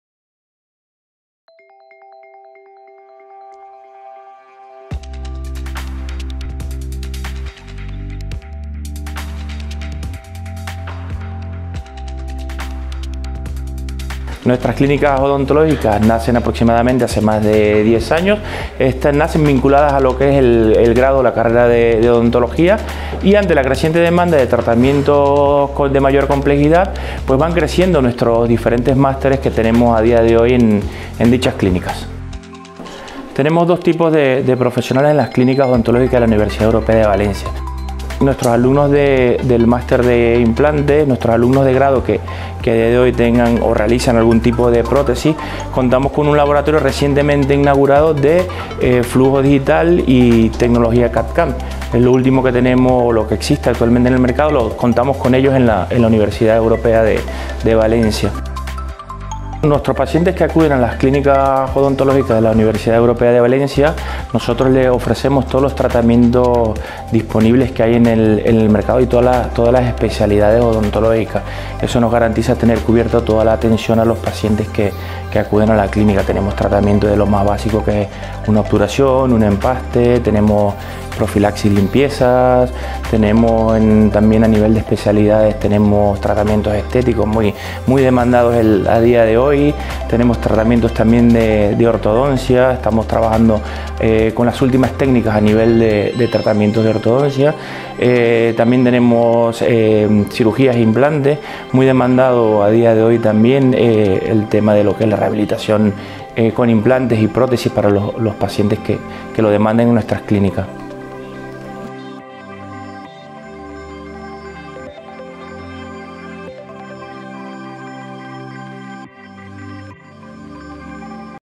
Clínicas Odontológicas UEV
La nueva Clínica Universitaria Odontológica de la Universidad Europea de Valencia pone a disposición del paciente las instalaciones más avanzadas y un amplio equipo de profesionales.
En la reciente Clínica Odontológica ubicada en la Calle Palleter 19-21 en Valencia, los estudiantes adquieren los conocimientos desde la práctica, a la vez que realizan una importante labor social, poniendo a disposición de los pacientes unas modernas instalaciones y un excelente equipo de profesionales y profesores en odontología especializada en tratamientos de Implantología, Ortodoncia, Ortodoncia Invisible, Prótesis dental, Periodoncia, Cirugía oral avanzada, Endodoncia y Estética dental. Confía en profesionales expertos.
Tu salud dental en las mejores manos con la Universidad Europea de Valencia.